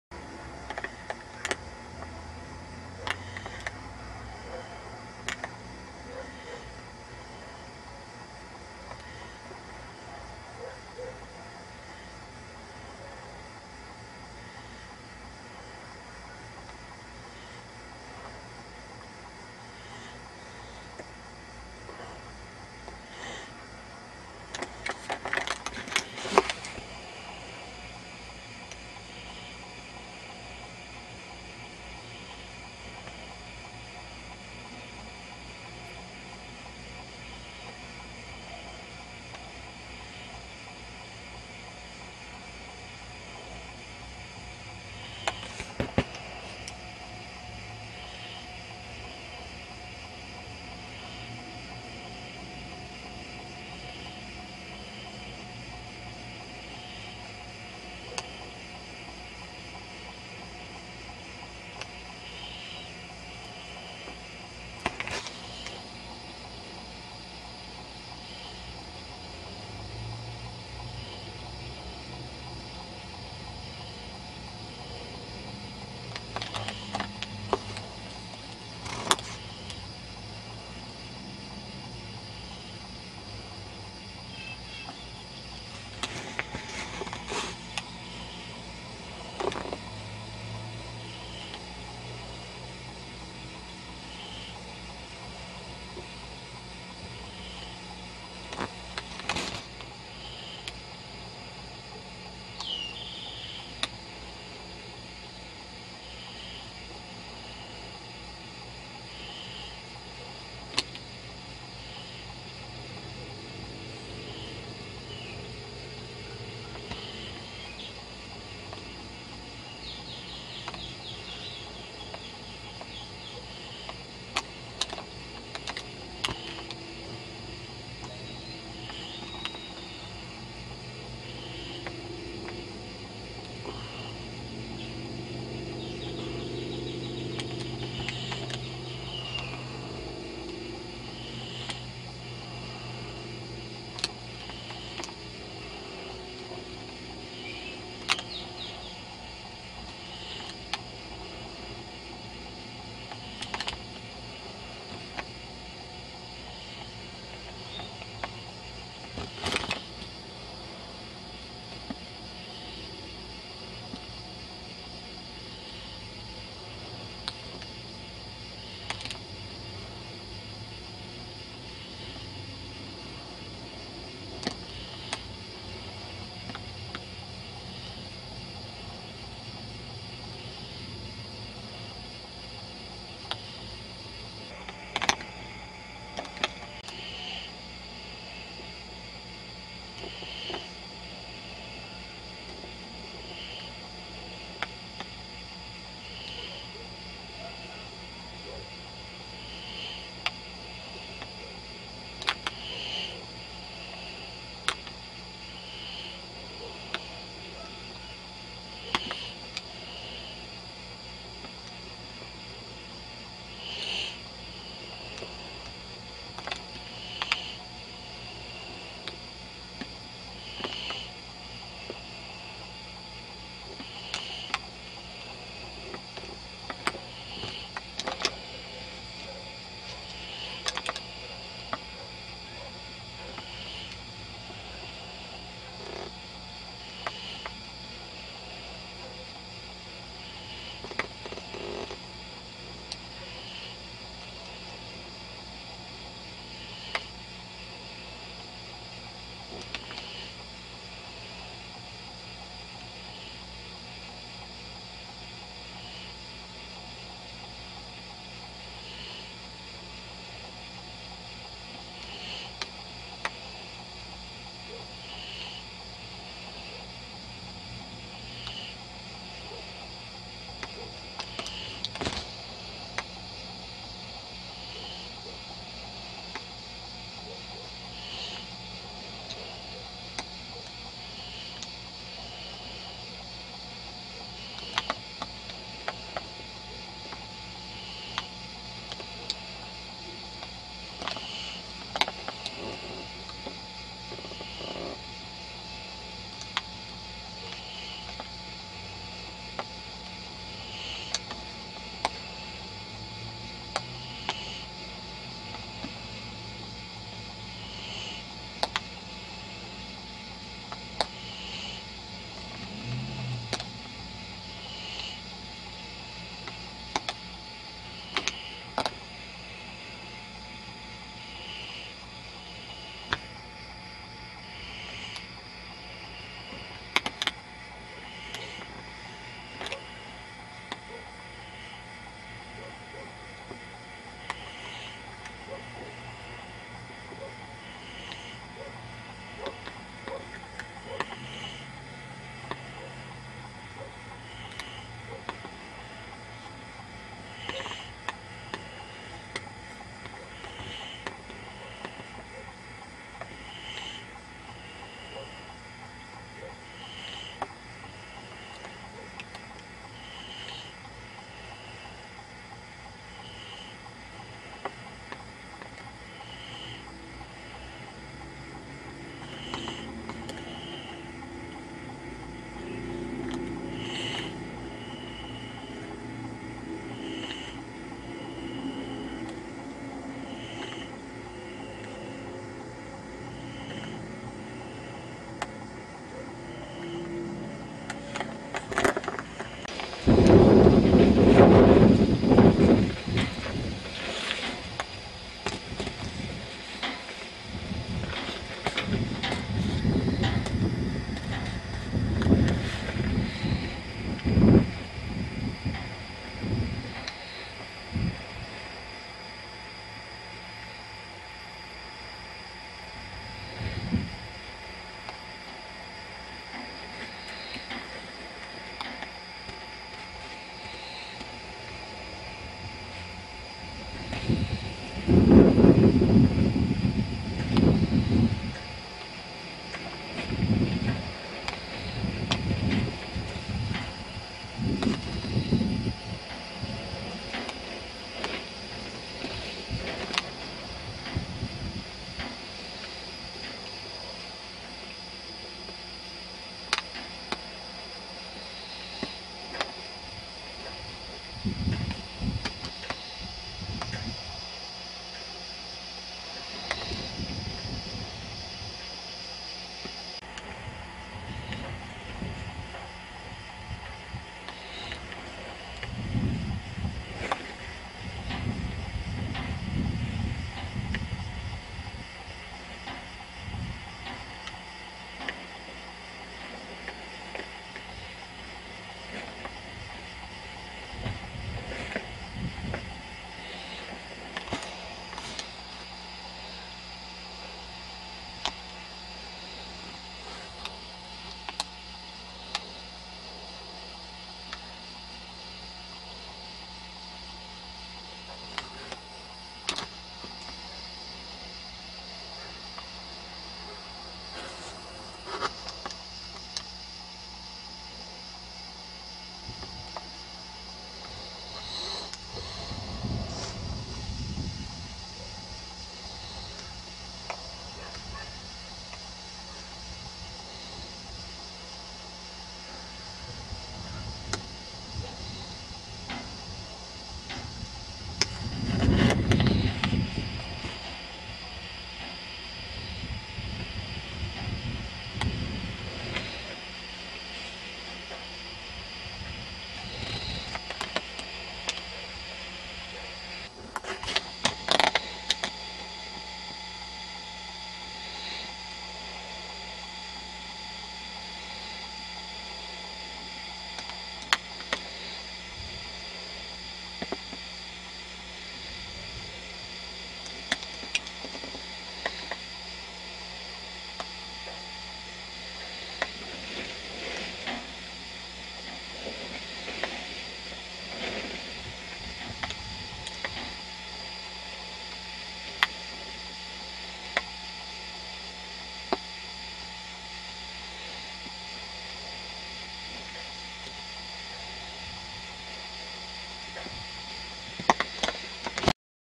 Multiple UFOs Artesia New Mexico May 1st 2009
Witness/Filmer Report.
May 1-3 2009
Artesia New Mexico
Several unknowns, twice two fly past each other.